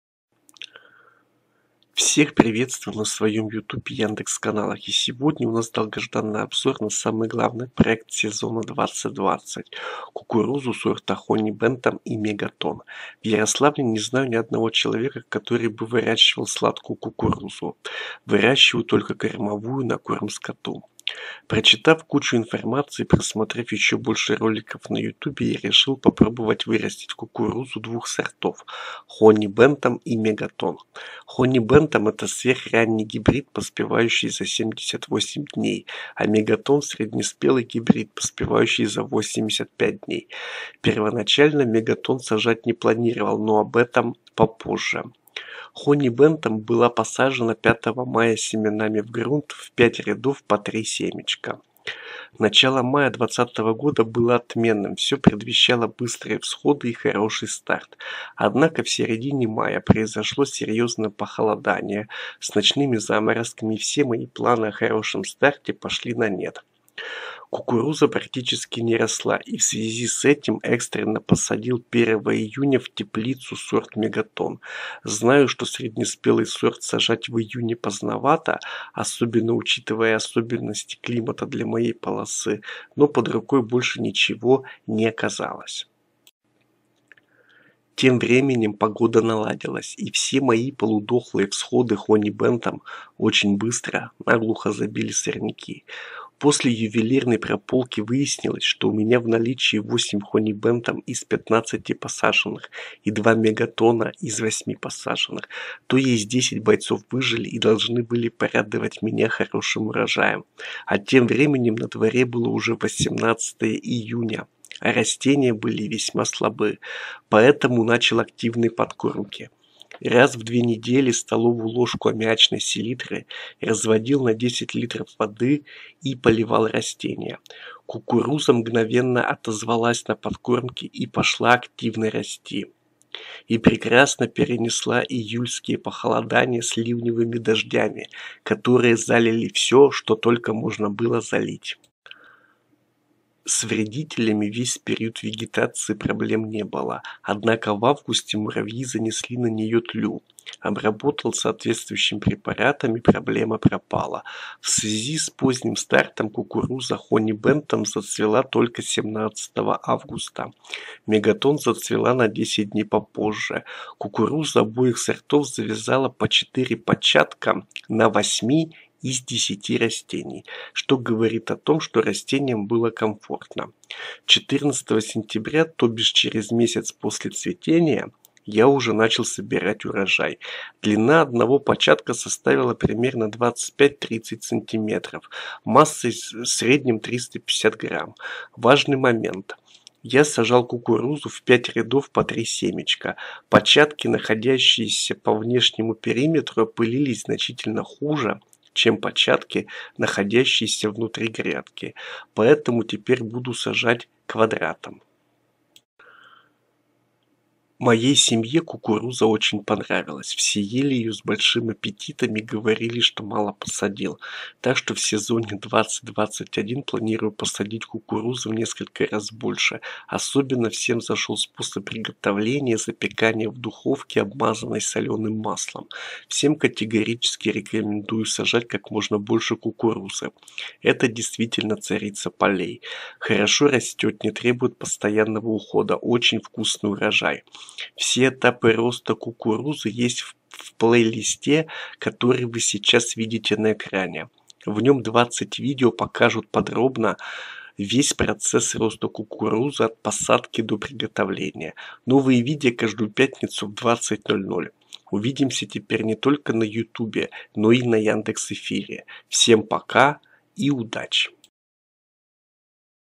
Главный успех сезона 2020. Обзор кукурузы. Хони Бэнтам F1 и Мегатон F1.
Поддержка канала